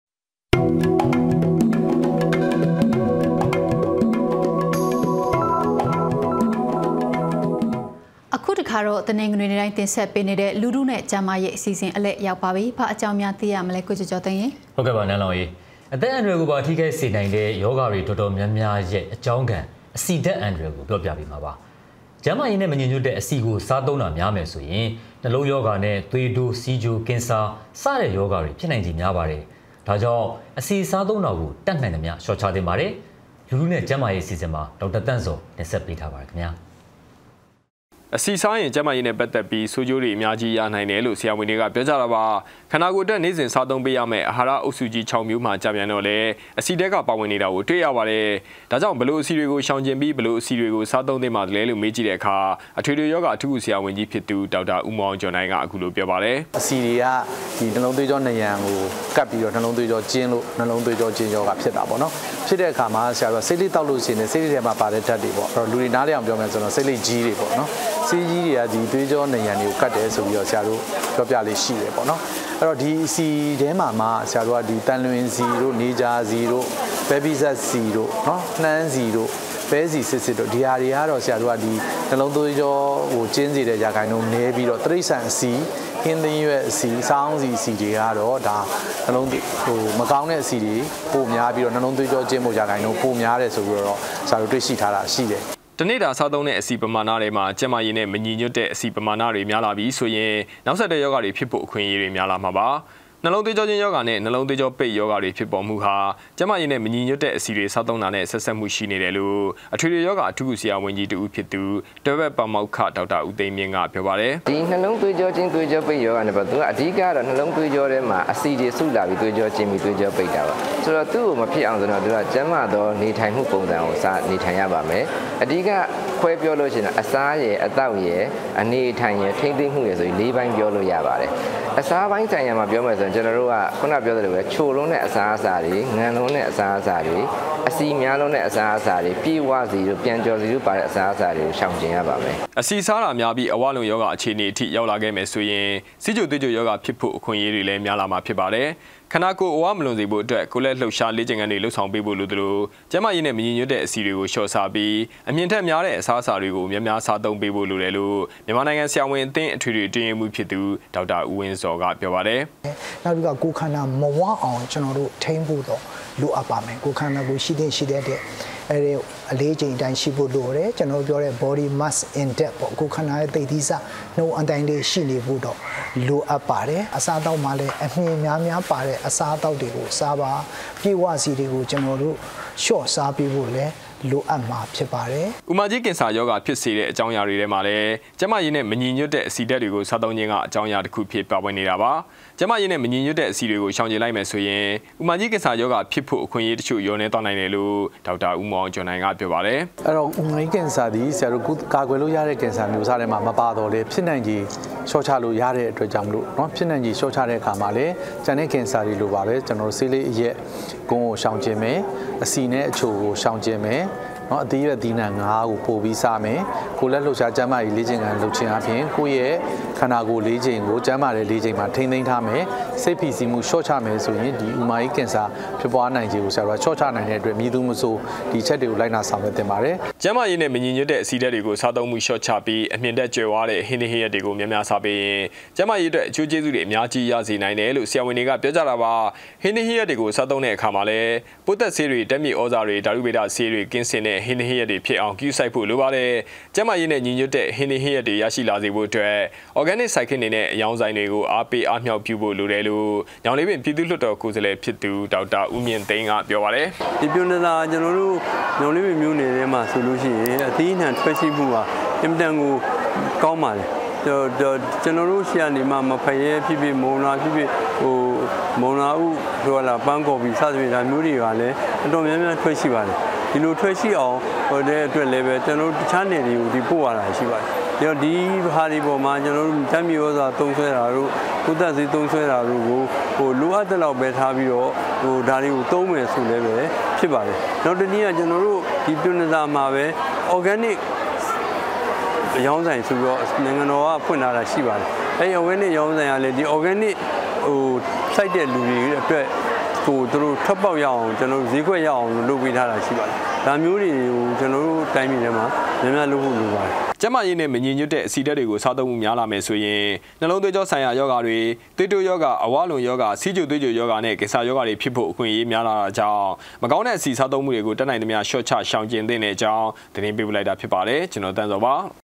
အဆီဓါတ္ကို ေရွာင္က်ဥ္ပါ
က်န္းမာေရးနဲ႔မညီညြတ္တဲ့ အဆီဓါတ္ စားသံုးမႈမ်ားမယ္ဆိုရင္ ႏွလံုးေရာဂါနဲ႔ ေသြးတိုး ဆီးခ်ိဳ ကင္ဆာ ဒီလို ေရာဂါေတြျဖစ္ႏိုင္ေခ် မ်ားပါတယ္။ ဒါေၾကာင့္ အဆီစားသံုးမႈကို တတ္ႏိုင္သမွ် ေလွ်ာ့ခ်သင့္ပါတယ္။ လူထုနဲ႔က်န္းမာေရးအစီအစဥ္မွာ ေဒါက္တာသန္႔ေဇာ္ တင္ဆက္ထားပါတယ္။